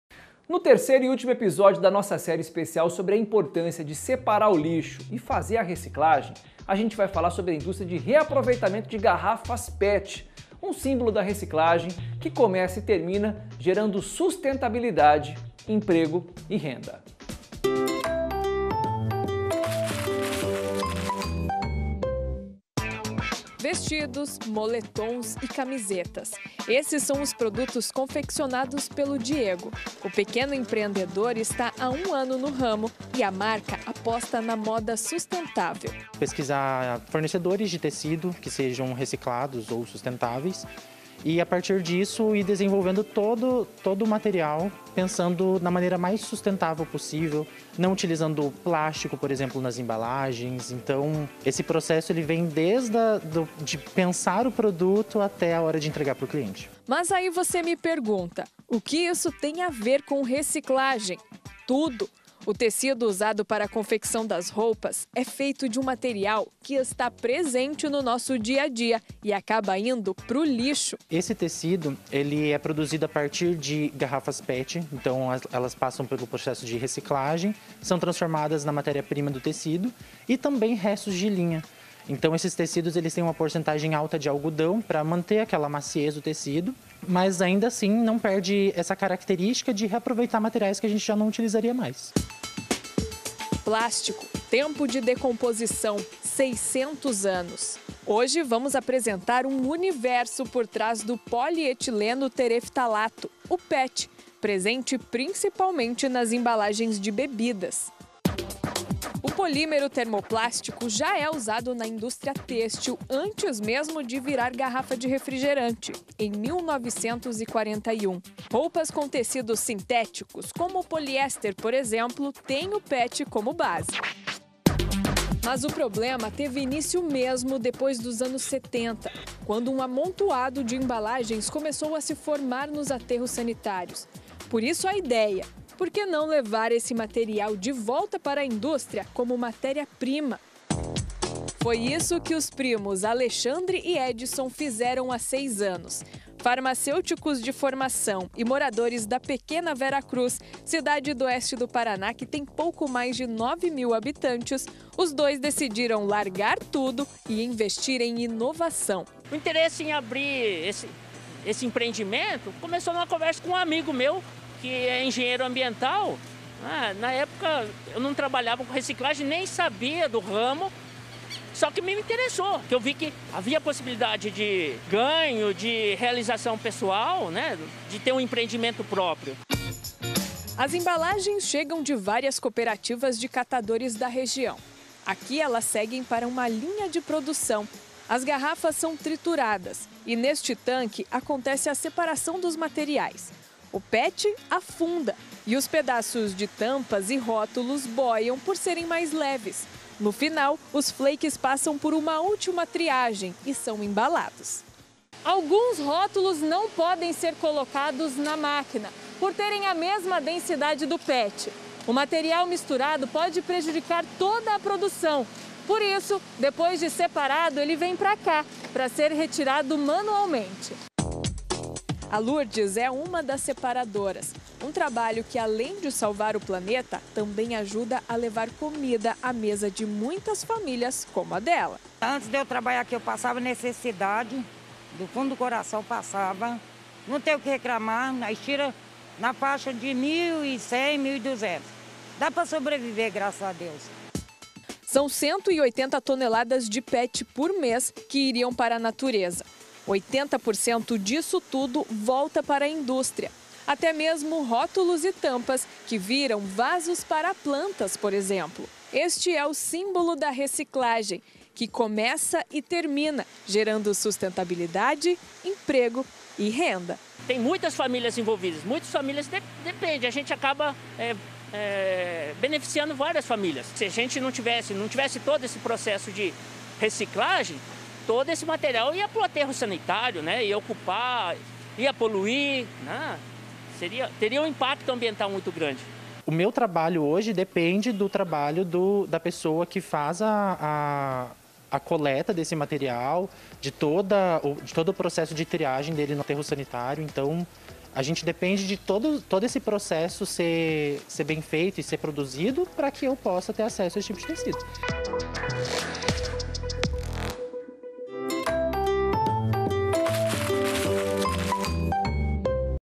Do lixo ao luxo: empreendedores buscam conhecimento e apostam em moda sustentável
No terceiro e último episódio da série sobre a importância da separação do lixo e reciclagem, vamos falar sobre a indústria de reaproveitamento de garrafas pet. Um símbolo da reciclagem que começa e termina, gerando sustentabilidade, emprego e renda. Confira todos os detalhes na reportagem!